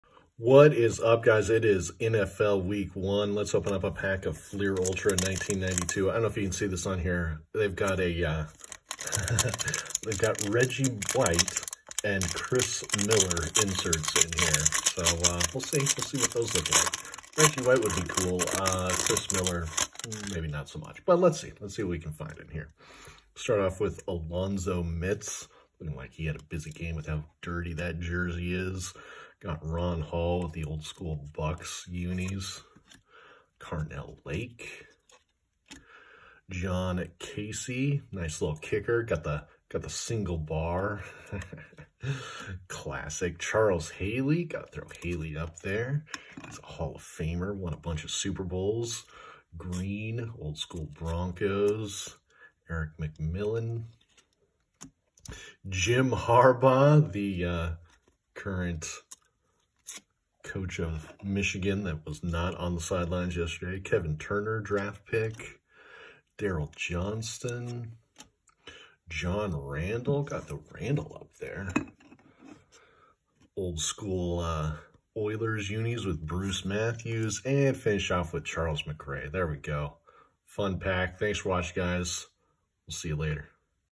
1992 Fleer Ultra NFL Pack #1
It’s week 1 of the NFL and this is pack 1 of 1992 Fleer Ultra😄